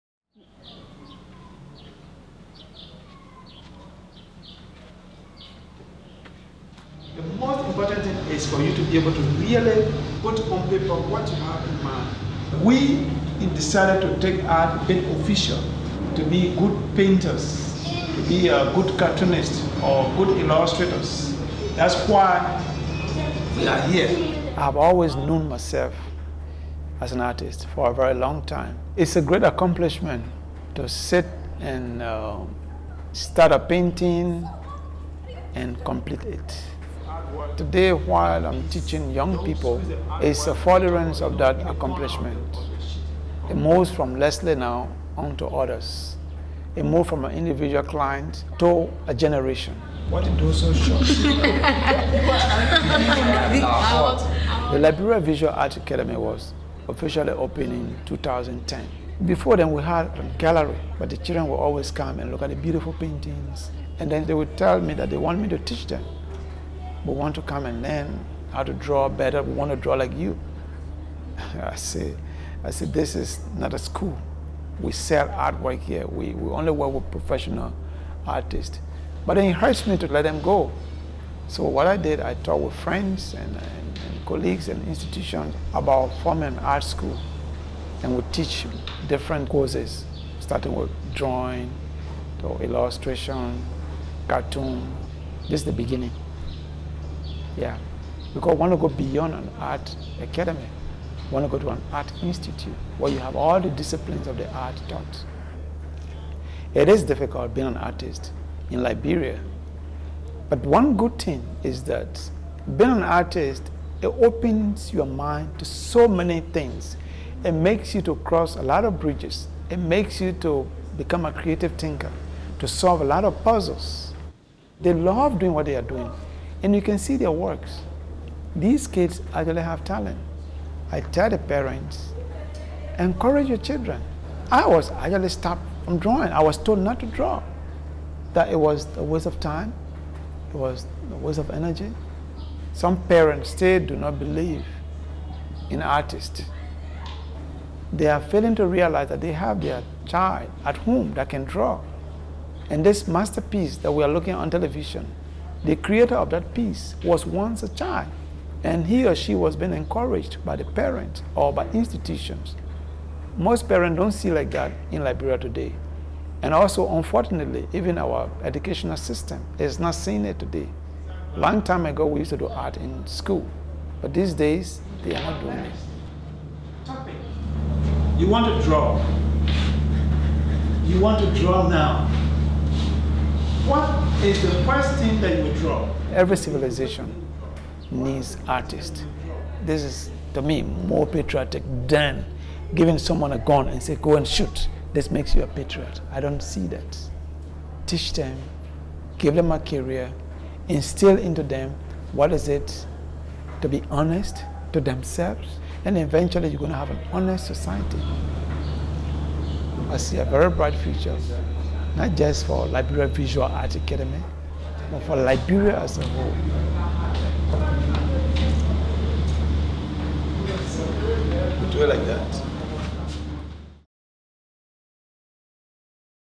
Accountability Lab's Partners: Liberia Visual Arts Academy
Liberian artist Leslie Lumeh founded the Liberia Visual Arts Academy (LiVArts) in Monrovia to offer a much needed arts education for talented young people. The Accountability Lab has partnered with Lumeh and LiVArts to create art-based integrity learning opportunities in Liberia. This film was produced by Jim Tuttle for Accountability Lab.